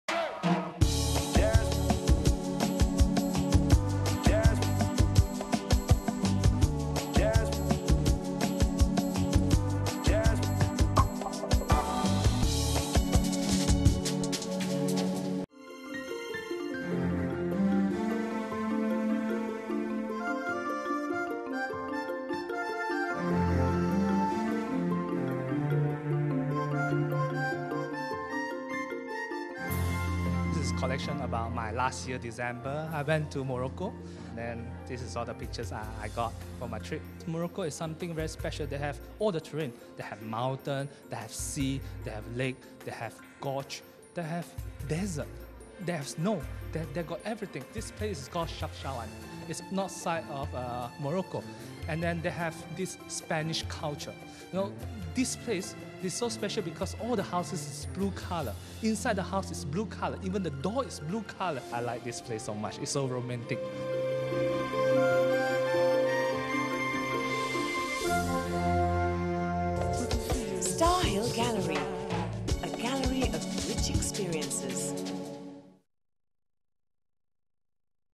Morocco -- Frozen in time
A photo exhibition by Jeffry Lim entitled 'Maroc' at Starhill Gallery.